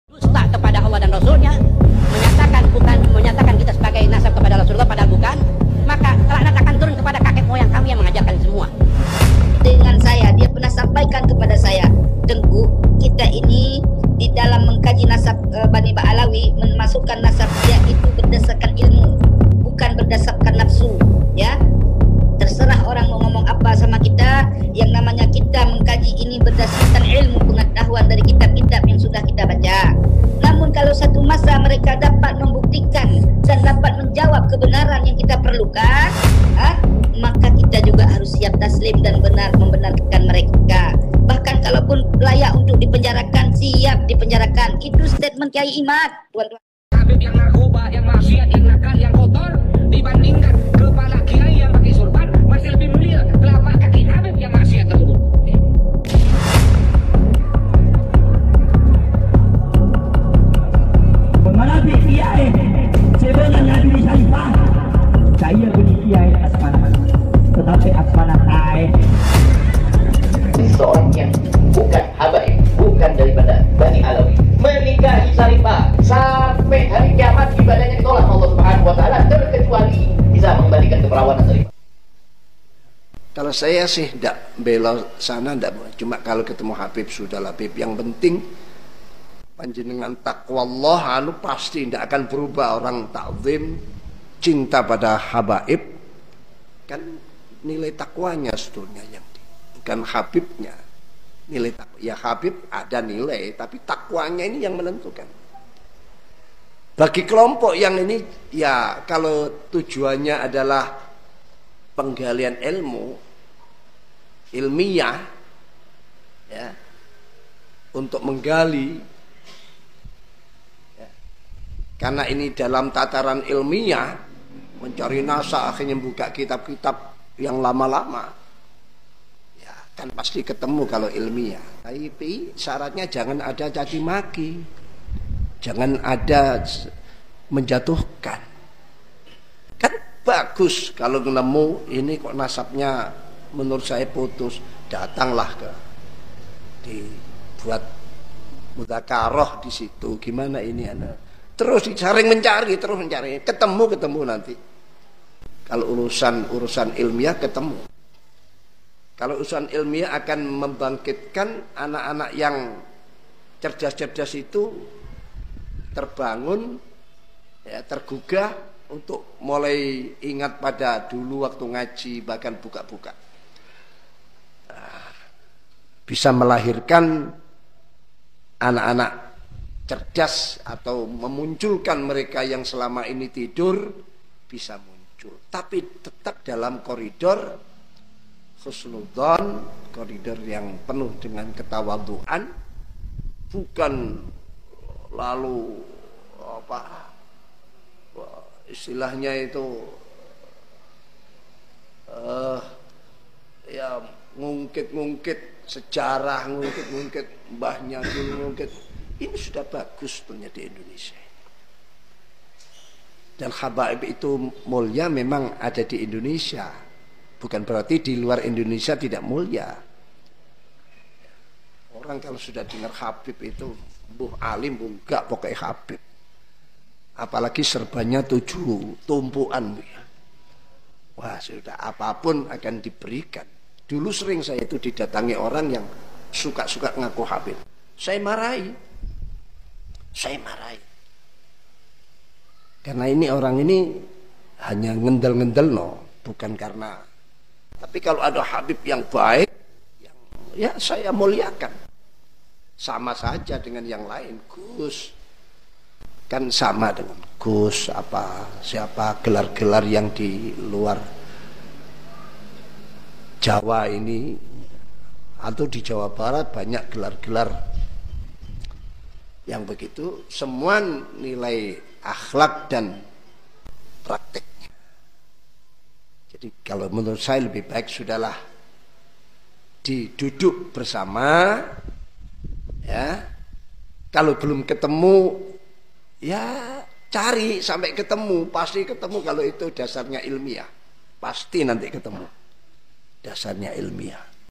Akhirnya Rois Am PBNU Mndukung Tesis Kyai Imad,Mudzakaro Solusi Polemik Nasab Tp Habib Nya Mau gk.?!
Akhirnya Rois Am PBNU Mendukung Tesis Kyai Imad, Saling Mudzakaroh Solusi Polemik Nasab Ini Tp RA / Habib Nya Mau gk..?!